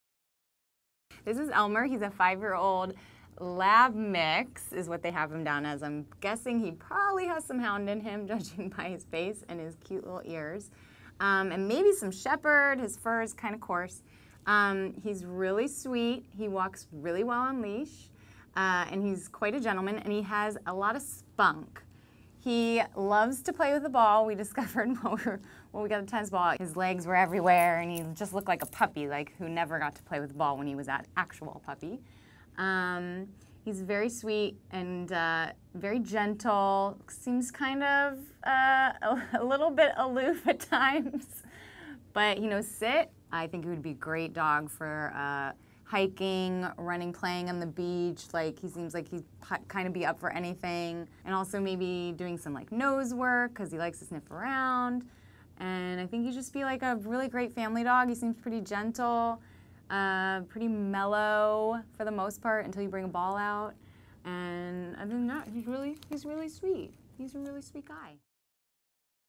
Adopt Elmer at NKLA
Meet Elmer a 5 yr old Labrador Retriever Mix.

For more information please:
visit: 1845 Pontius Ave. Los Angeles, CA, 90025